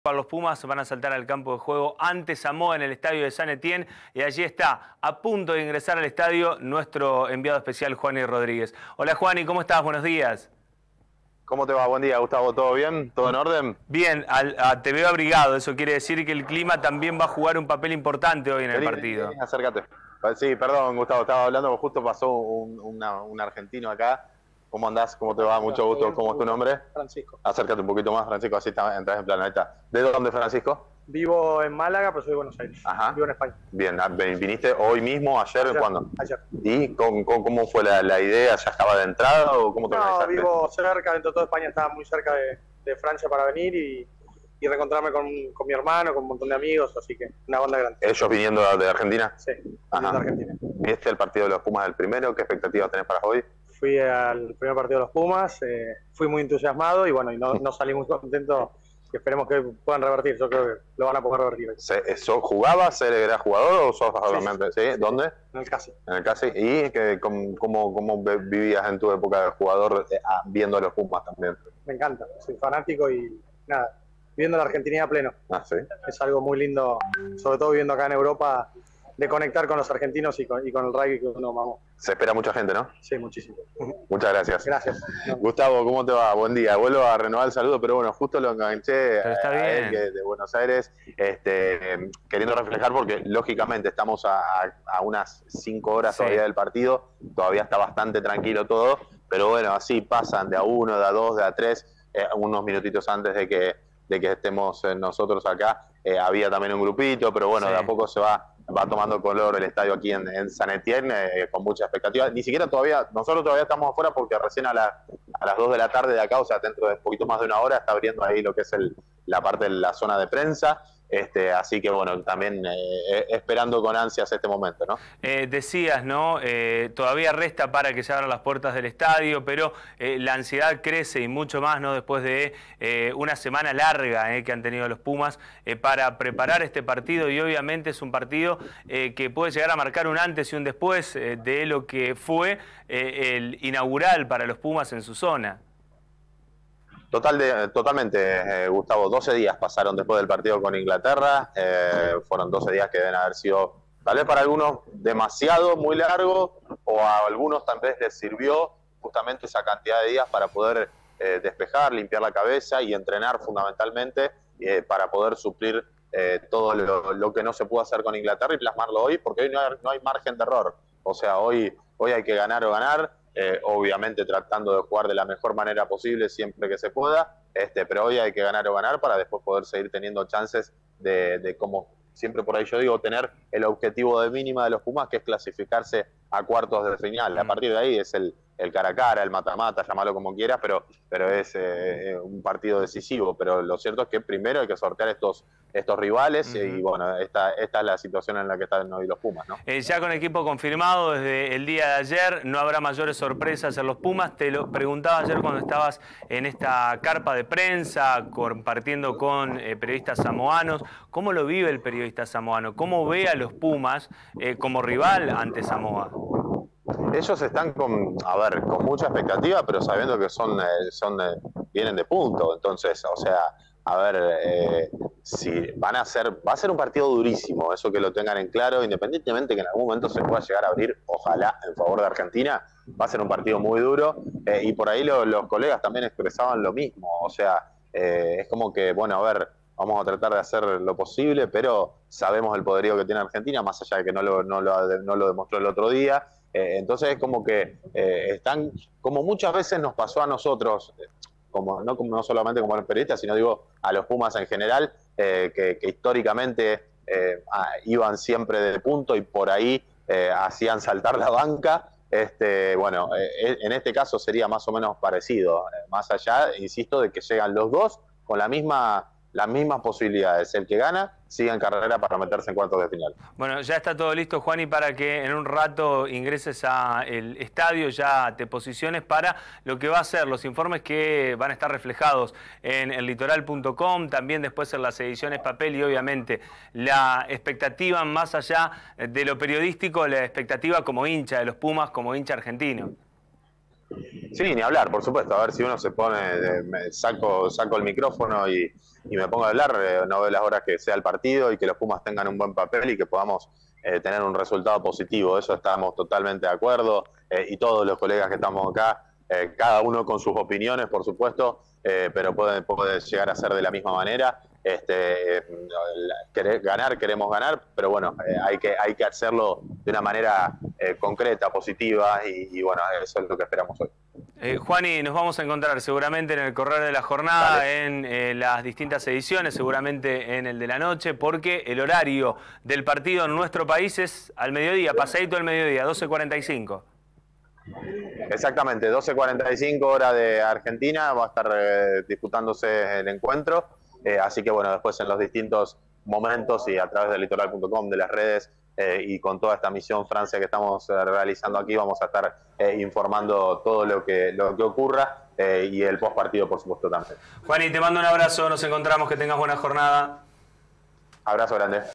LOS PUMAS VAN POR EL PRIMER TRIUNFO EN EL MUNDIAL DE RUGBY//JUANI RODRÍGUEZ DESDE FRANCIA
LOS PUMAS VAN POR EL PRIMER TRIUNFO EN EL MUNDIAL DE RUGBY//JUANI RODRÍGUEZ DESDE FRANCIA - JUAN IGNACIO RODRIGUEZ